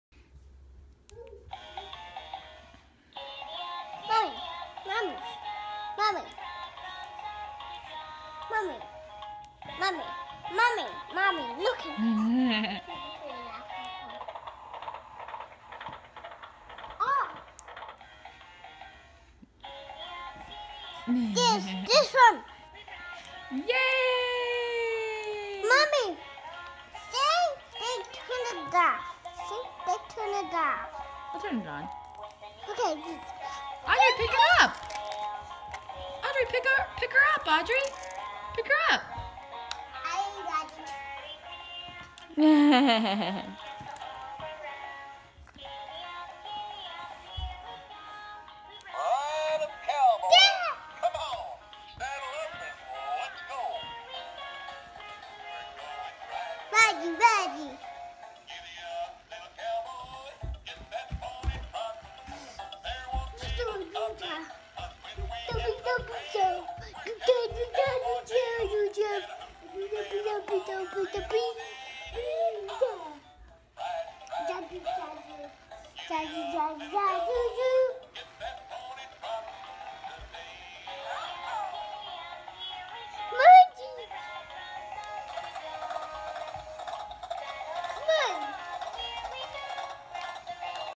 Audrey dancing with her horse
For the grandparents and aunts